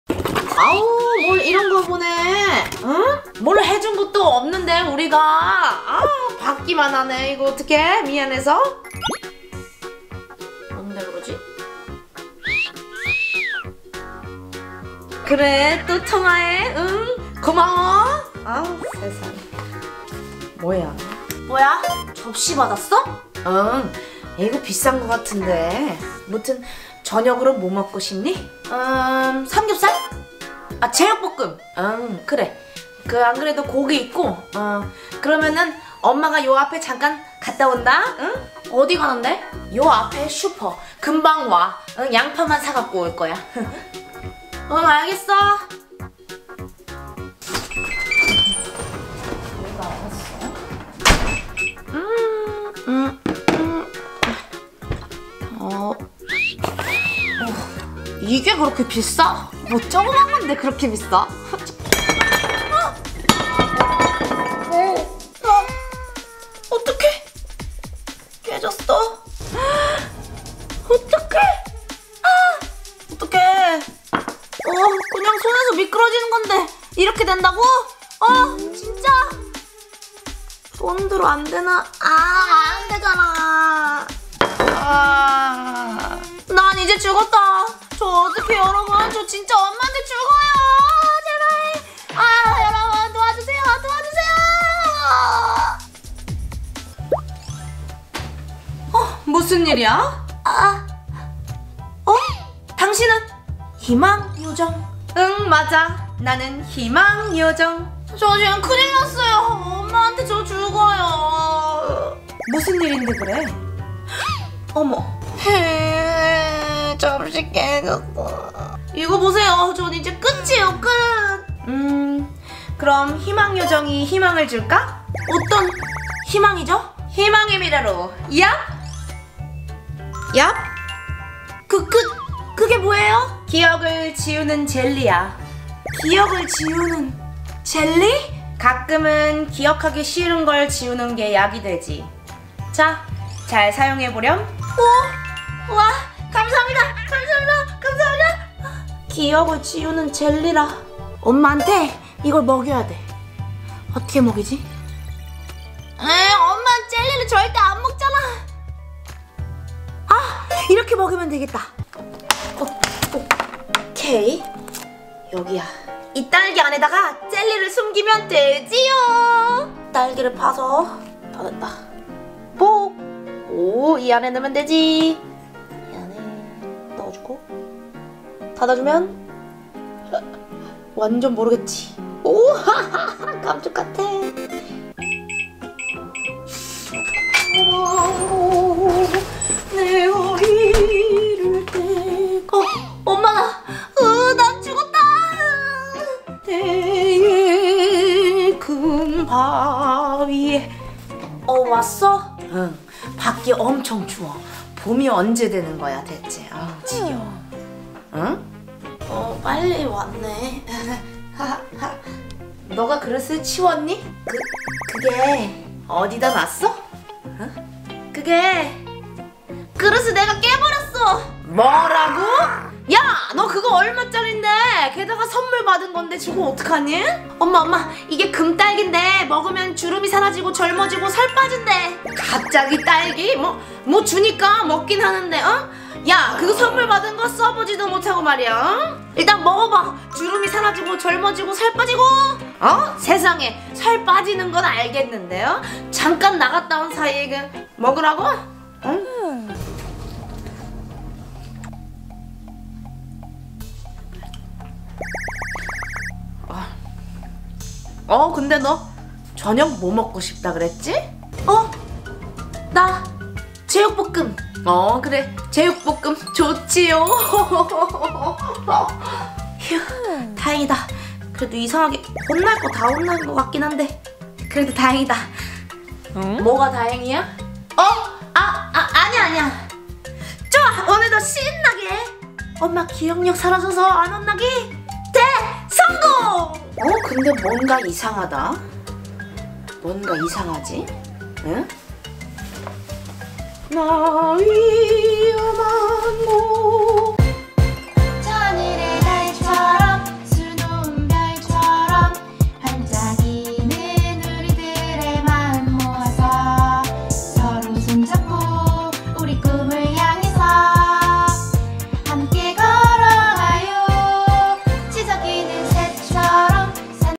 엄마한테 혼나게 생긴 오비키 과연 위기를 극복할 수 있을지?!
마침 엄마가 받은 선물을
와장창 깨버림 ㅠㅠ